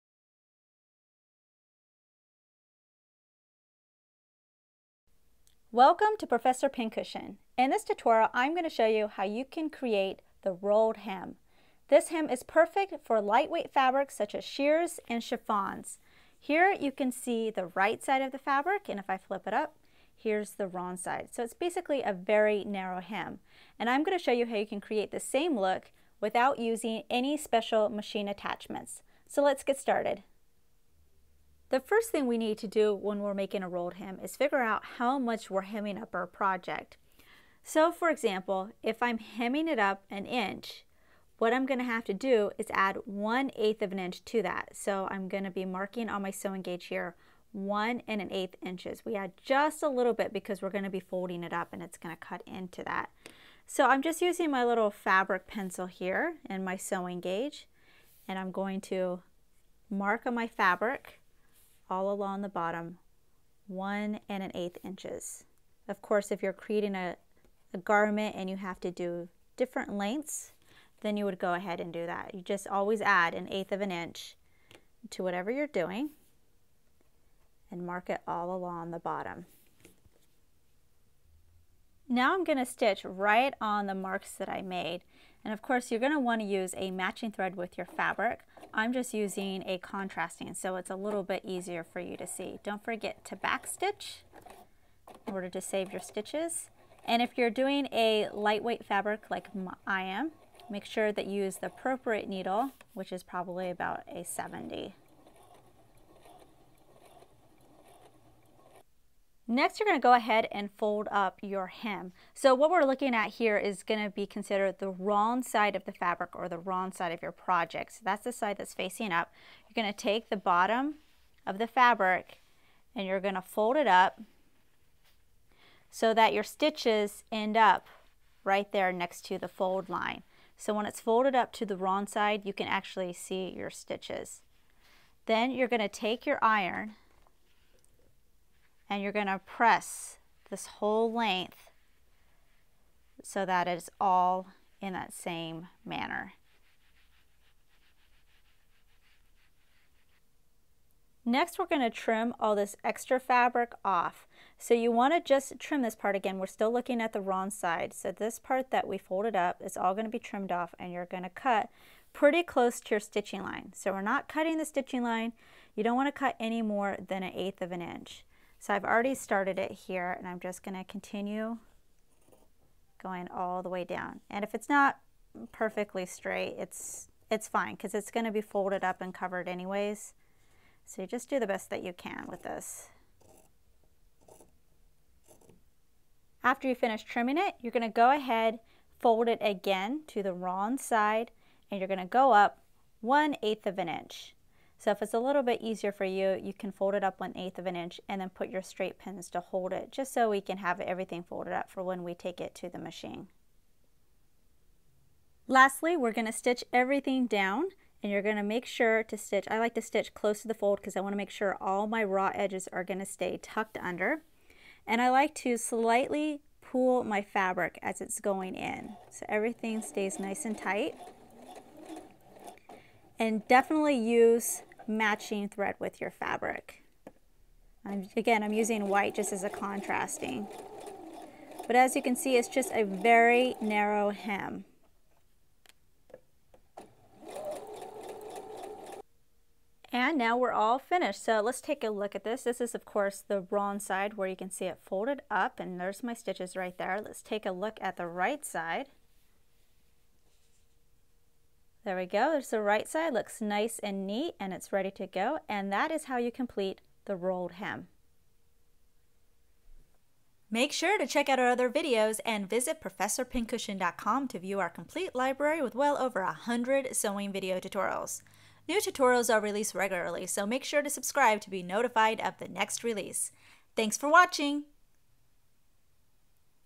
How To Sew A Rolled Hem
A perfect hem for chiffon and silk, learn how to create a the elegant rolled hem without a special machine foot attachment.